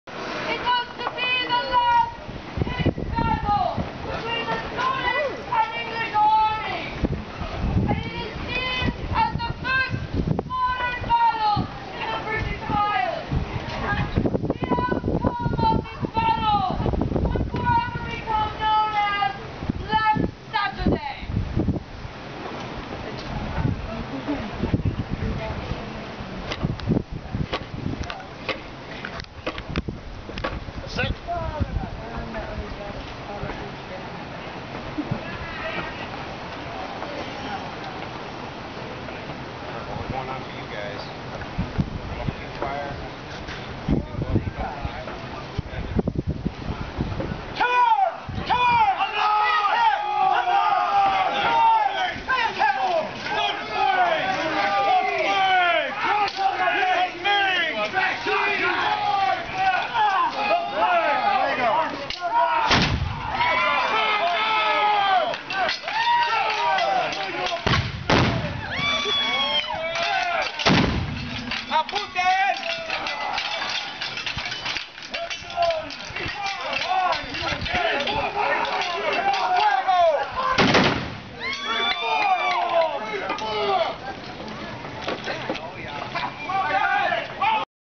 La Orden de Santiago at Koroneberg RenFest battle 2013 pt. 1
Canons and muskets firing during the battle pagent during the Sunday battle of the 4th weekend at the Koroneberg renaissance festival in Corona California, 2013.